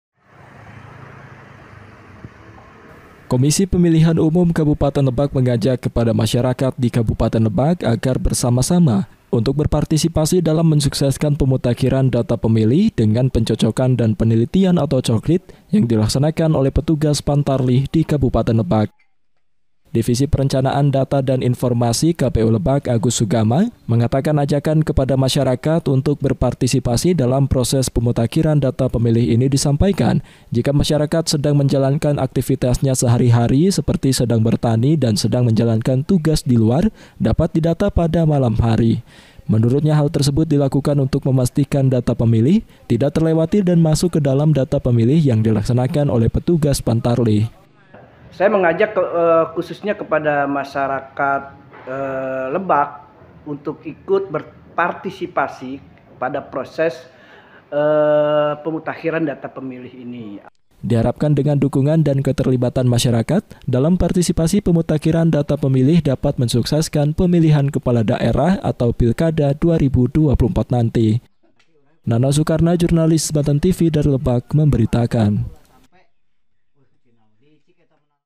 KPU LEBAK AJAK MASYARAKAT IKUT SUKSESKAN COKLIT
Dalam rangka mensukseskan pemutakhiran data pemilih dengan pencocokan dan penelitian atau Coklit, KPU Lebak mengajak kepada masyarakat di Kabupaten Lebak untuk bersama-sama mengawal dan mensukseskan Coklit yang dilaksanakan oleh petugas Pantarlih.